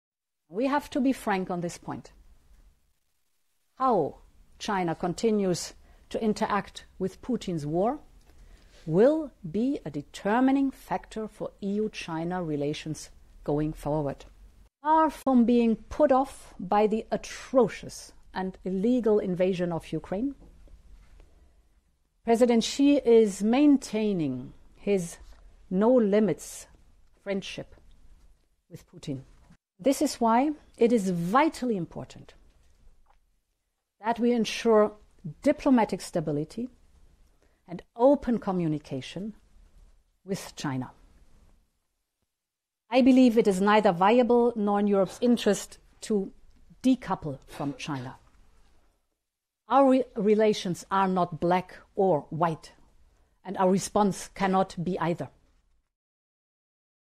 Von der Leyen: Verhältnis zu China abhängig von Haltung zu Putin | AFP
Kommissionspräsidentin Ursula von der Leyen hat die Beziehungen der EU zu China von der Haltung Pekings im Ukraine-Krieg abhängig gemacht. "Wie China in Zukunft mit Putins Krieg umgeht, ist ein bestimmender Faktor", sagte von der Leyen vor einer Reise nach Peking.